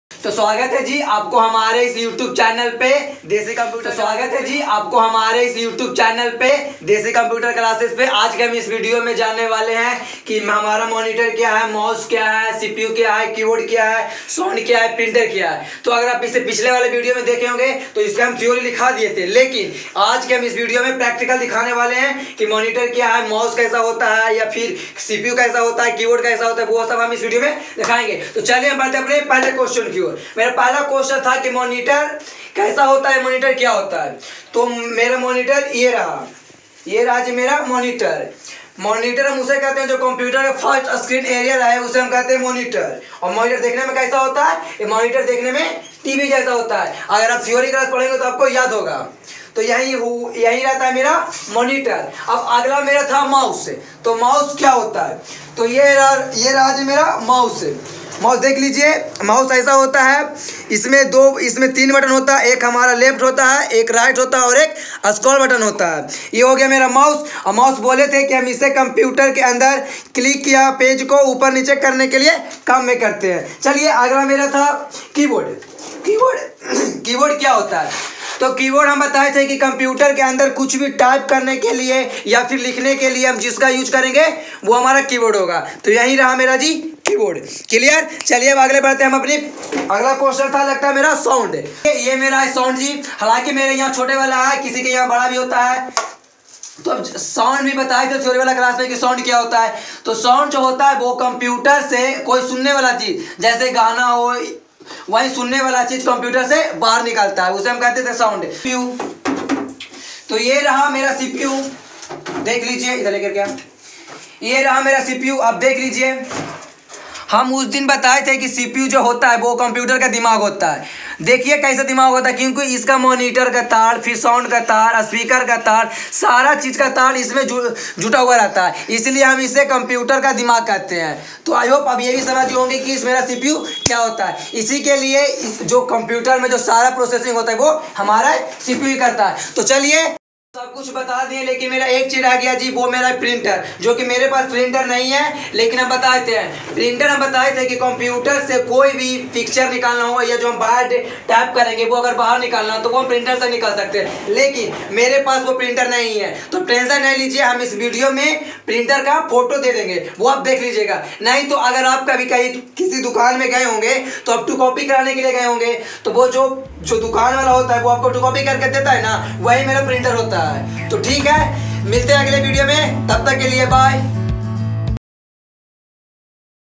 "Zero se Hero" batch Day -3 ka Practical work
hello guys
aapko hum is video me btane wale hai ki
moniter kya hai
mouse kya hai
keyboard kya hai
sound kya hai
with pratical work